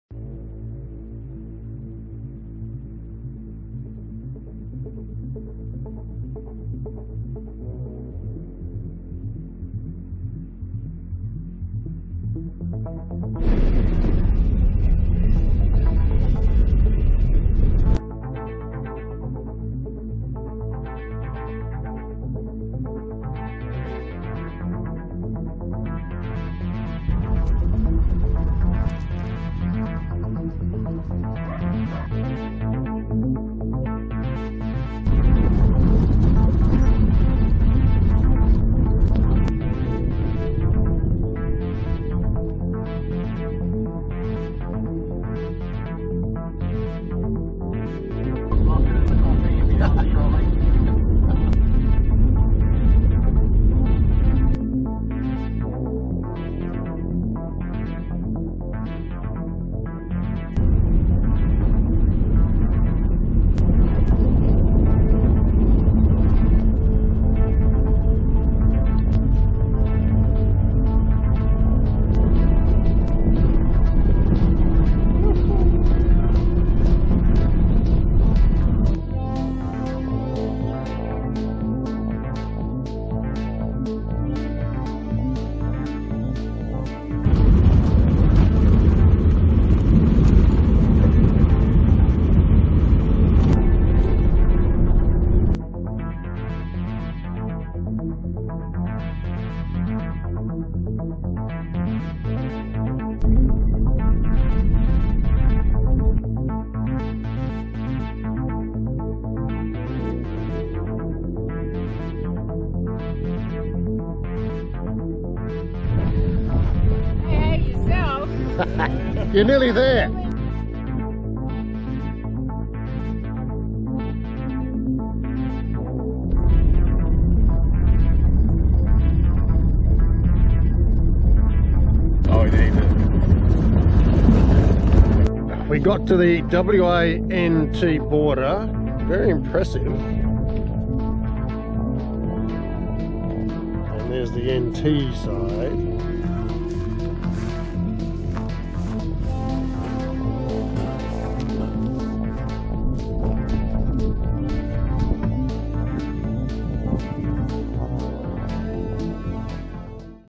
Tanami Road - Western Australia 300km Section
A quick look at the WA section  of the Tanami Road (300km's)
The Tanami Road runs from Halls Creek in WA to Alice Springs in the NT